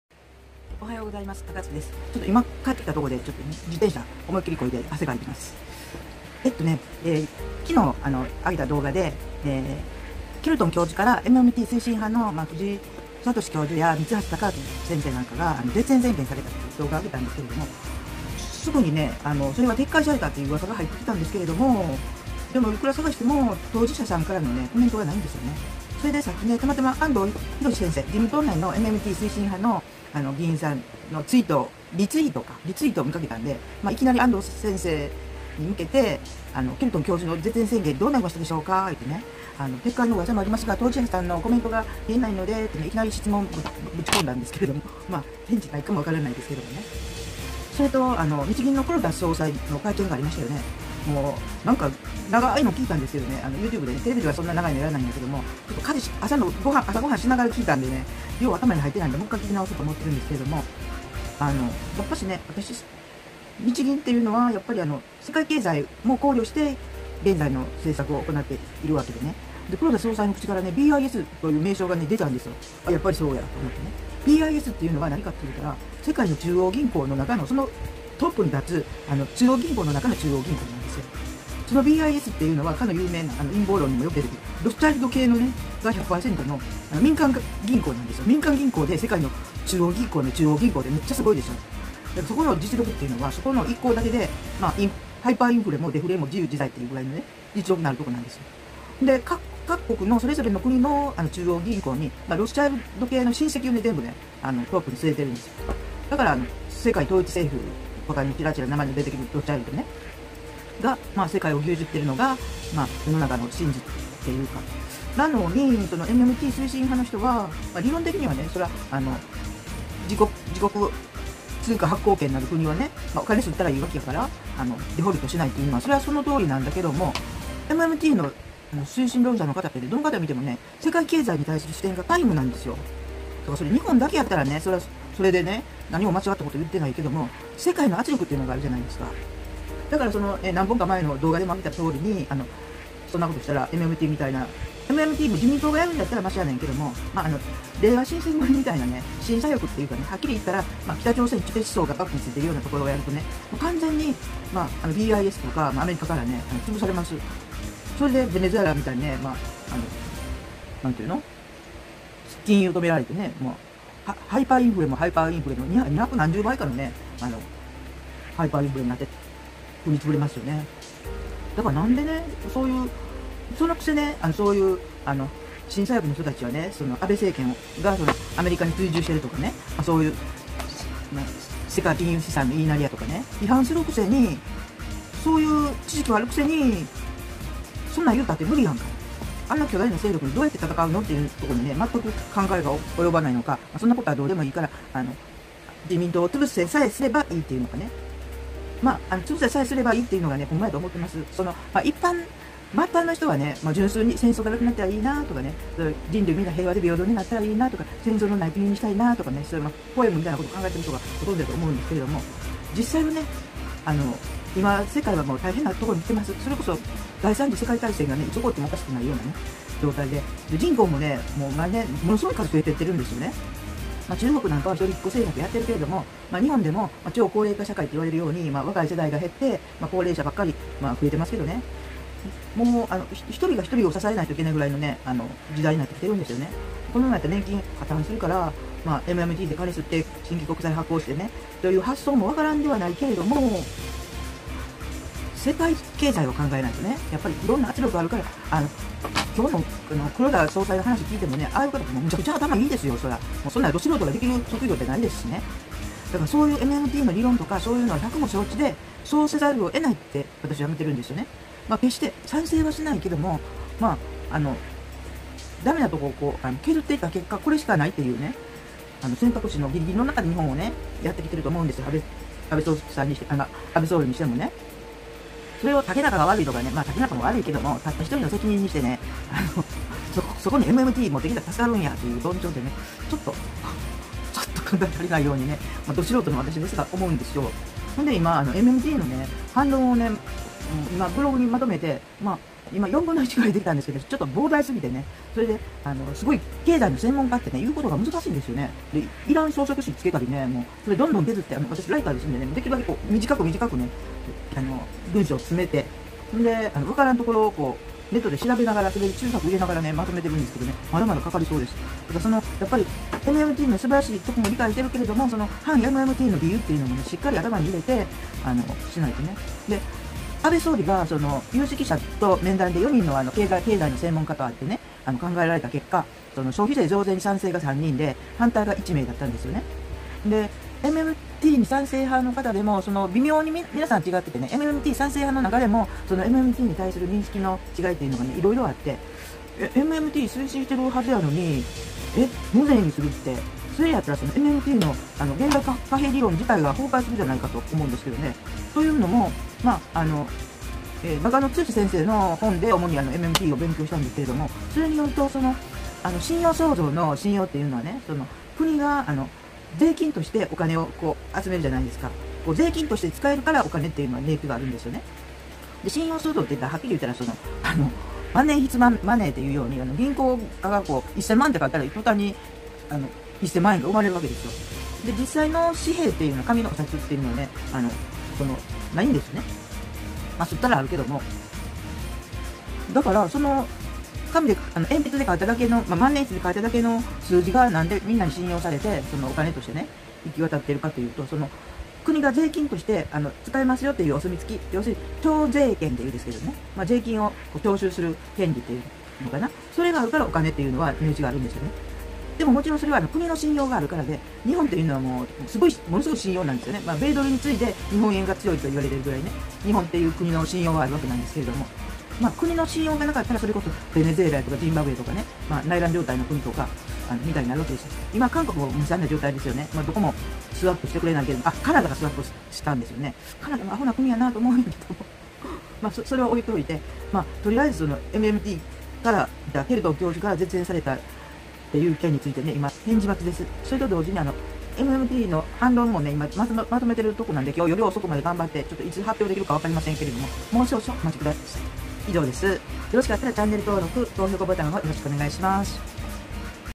ケルトン教授の絶縁宣言から撤回？当事者のコメントがないけど？
前の動画で日本シンポジウムを終えたケルトン教授から、藤井聡教授や三橋貴明氏、自民党のMMT推進派の西田議員安藤議員らと絶縁宣言がされました。
それは撤回？のうわさもあるけど、当事者のコメントがないので直接質問しています。
レイ教授の講演は8月から予定されていたし日本出版もあり予定とおり開かれるようですが。